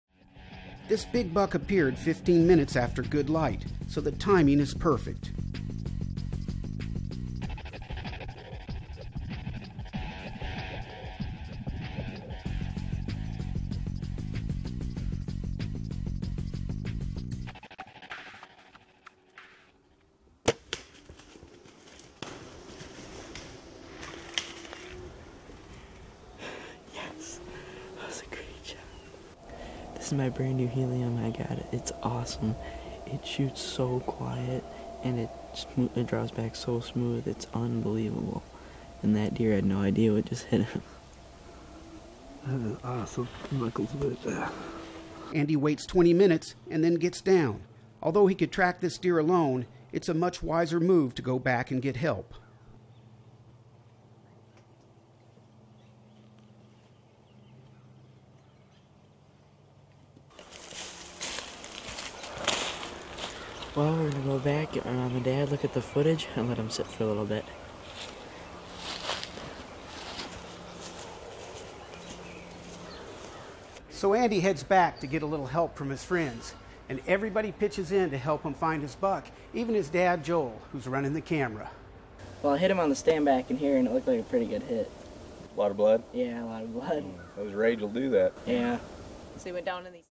Teen Hunting Whitetail Deer with Bow in Kansas
Join Joel, Janice and Andy Maxfield on a family whitetail bow hunt in Kansas.
5261